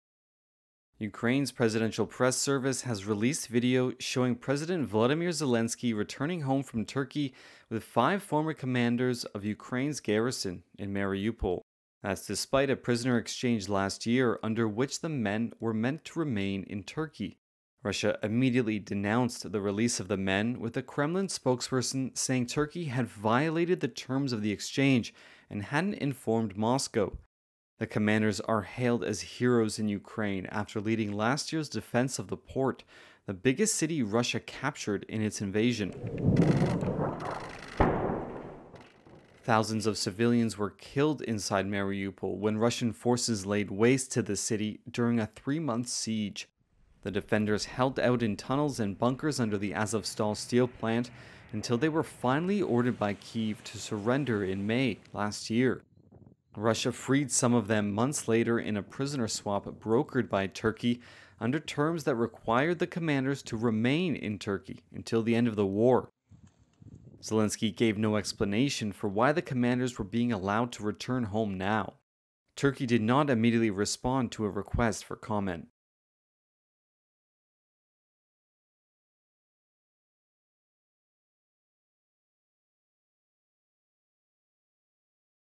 Zelensky brings home Azovstal steel plant commanders who were released to Turkey
Ukrainian President Volodymyr Zelenskiy, returning home from a visit to Turkey, brought with him five commanders of Ukraine's former garrison in Mariupol, forced to live in Turkey under the terms of a prisoner exchange last year.

The commanders, lionised as heroes in Ukraine, led last year's defence of the port, the biggest city Russia captured in its invasion. Thousands of of civilians were killed inside Mariupol when Russian forces laid the city to waste during a three-month siege.

The Ukrainian defenders, who held out in tunnels and bunkers under a steel plant, were finally ordered by Kyiv to surrender in May last year. Moscow freed some of them in September last year in a prisoner swap brokered by Ankara, under terms that required the commanders to remain in Turkey until the end of the war.

"We are returning home from Turkey and bringing our heroes home," said Zelenskiy who met Turkish President Tayyip Erdogan for talks in Istanbul on Friday.

"Ukrainian soldiers Denys Prokopenko, Svyatoslav Palamar, Serhiy Volynsky, Oleh Khomenko, Denys Shleha. They will finally be with their relatives," he said on the Telegram messaging app.

Zelenskiy gave no explanation for why the commanders were being allowed to return home now. Turkey's Directorate of Communications did not immediately respond to a request for comment. There was no immediate comment from Moscow.

Zelenskiy posted a one-minute video showing himself and other officials shaking hands and hugging the smiling commanders before they boarded a Czech airplane together.